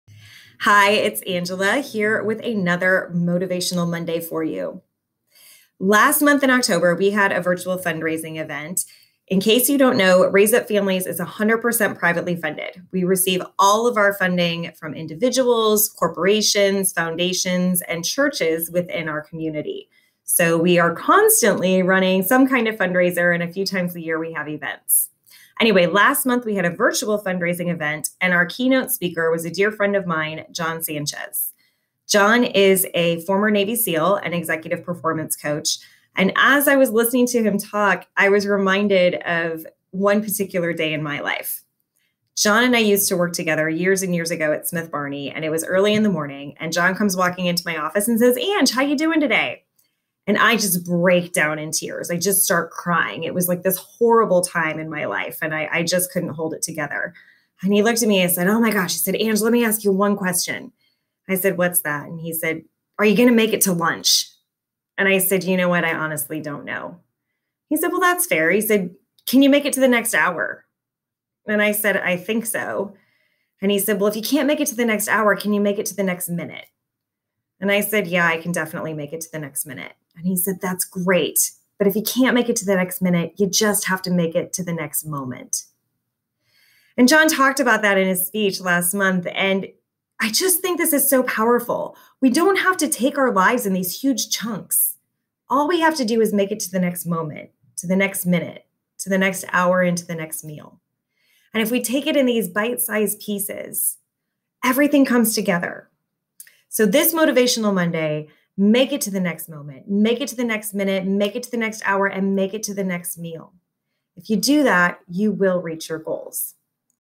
Make it to the next moment
We don't have to take our lives in these huge chunks. All we have to do is make it to the next moment.  If we can take it in these bite size pieces, everything comes together. Make it to the next moment, the next minute, the hour, the next meal.  If you do that, you will reach your goals.